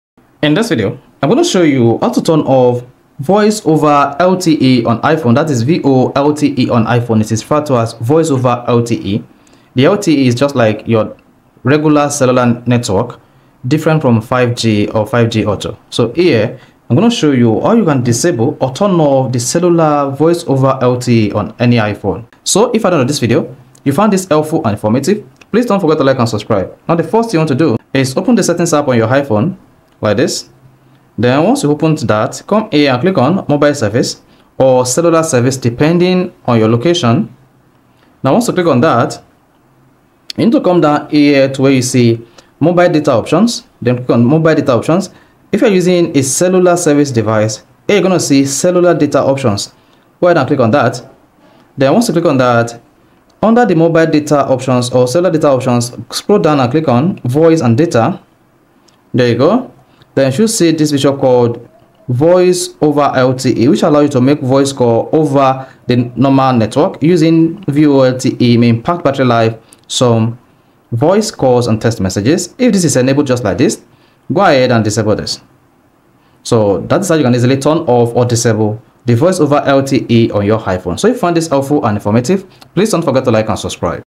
How to Turn Off Voice Over LTE on iPhone
Here, you will learn how to turn off voice over LTE on iPhone. With this, you will not be able to use internet access while receiving regular call on your phone.